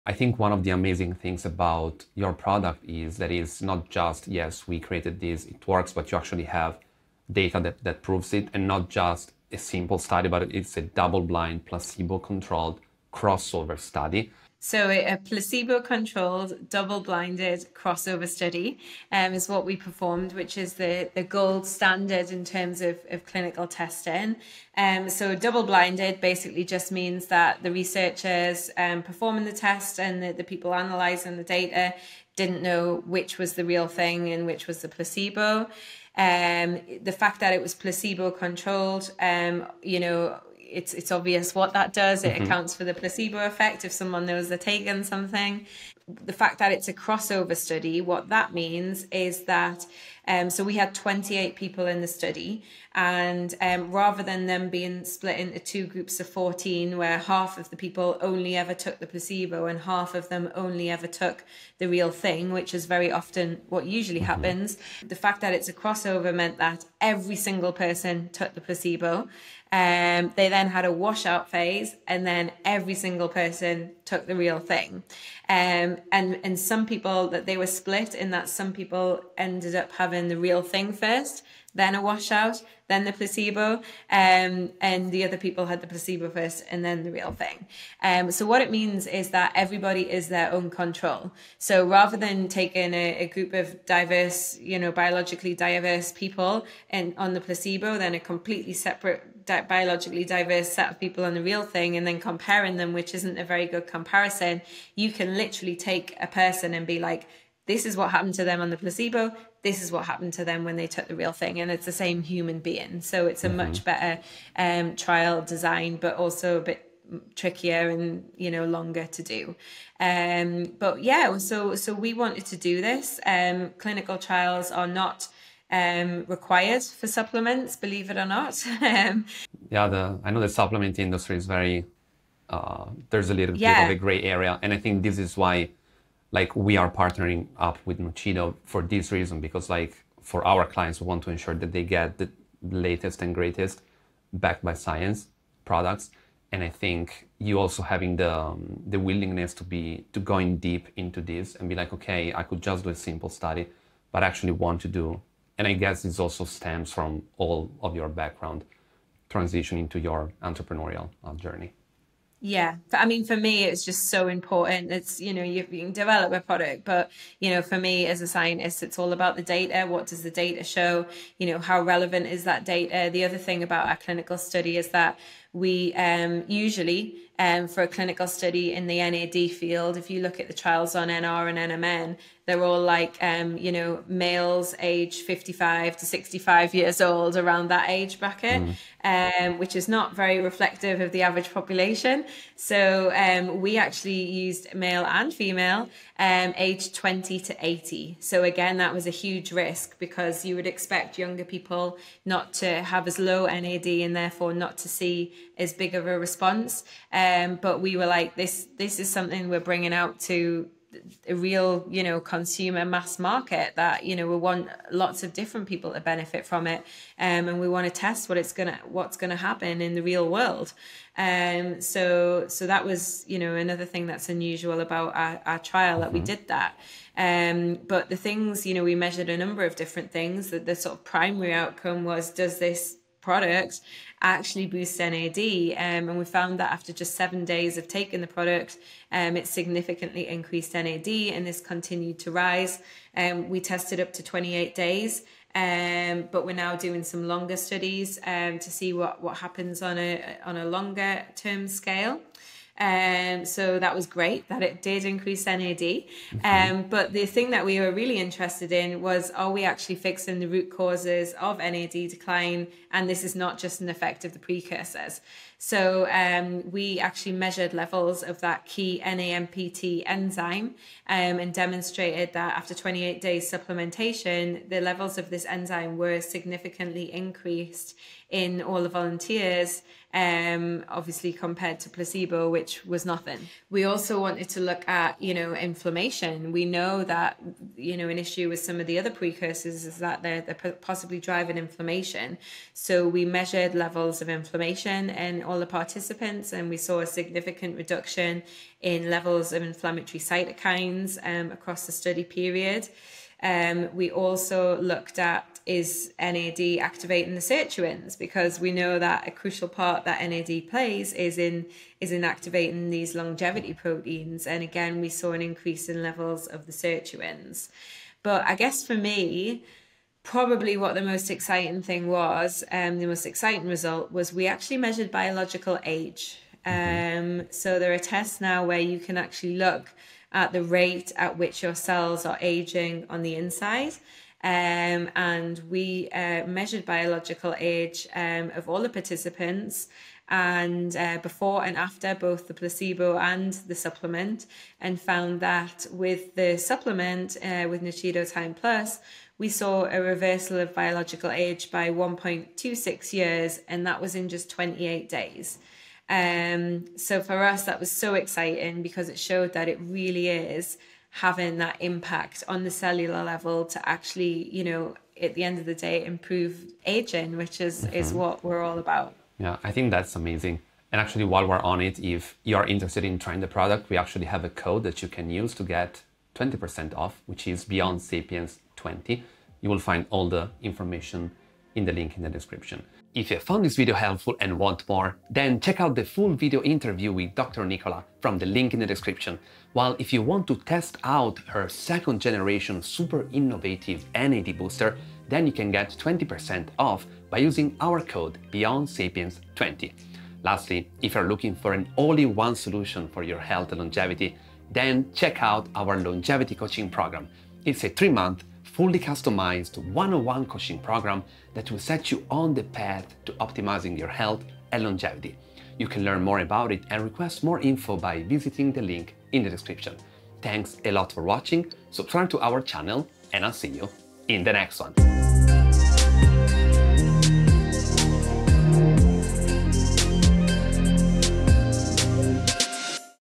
This Supplement Increases NAD+ by 242% (Clinical Trial Preview) | Longevity Nugget
In this interview with Dr. Nichola Conlon, founder and CEO of Nuchido Laboratories, we shed light on nuchido Time+, the innovative NAD+ booster, and its clinical evidence. 🥼

We invite you to join us in this video to gain an in-depth understanding of this important topic. Just like us at Beyond Sapiens, Dr. Nichola Conlon is on a mission to democratize science, helping people reverse their biological age and improve their healthspan.